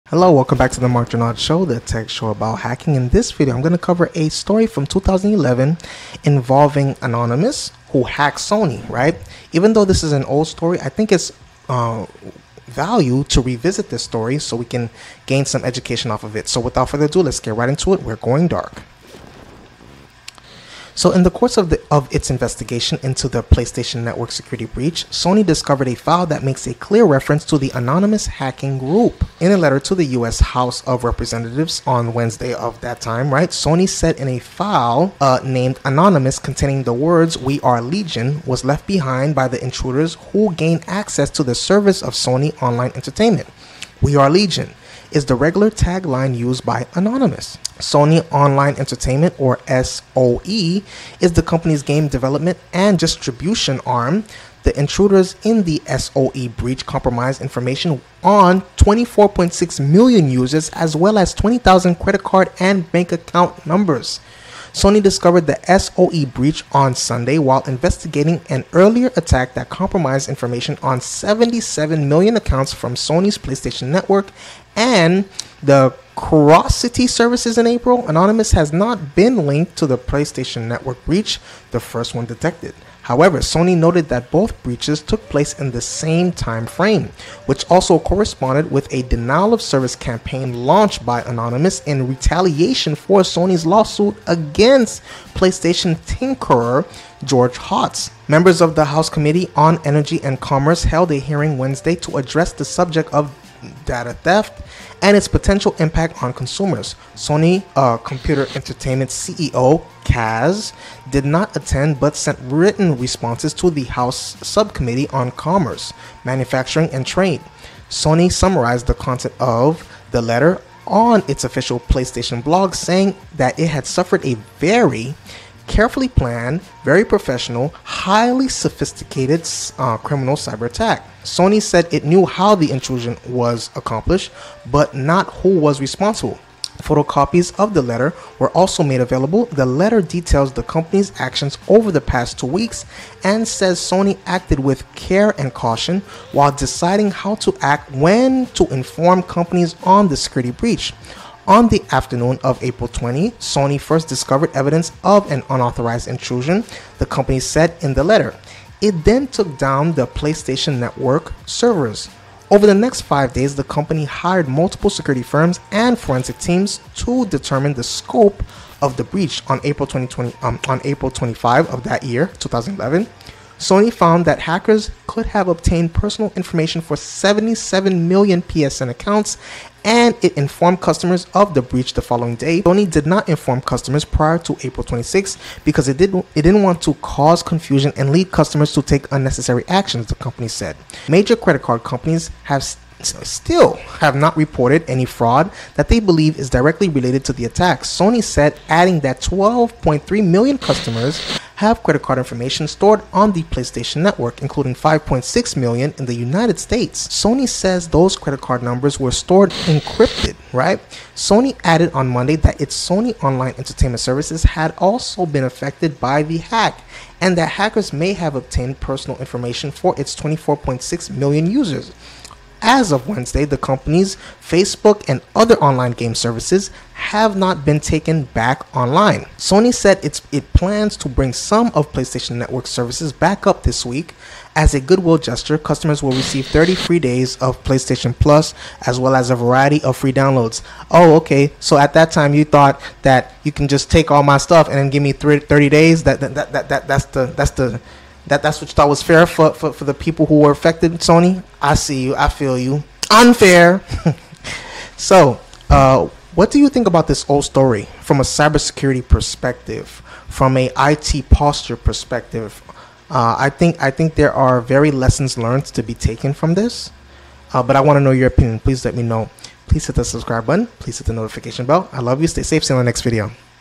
Sony Attacked Anonymous And Immediately Regretted That
Welcome Back Hack Pack!

In my latest video, I revisit a story from 2011 when the Tech giant Sony went after Anonymous and the responded action was highly unexpected.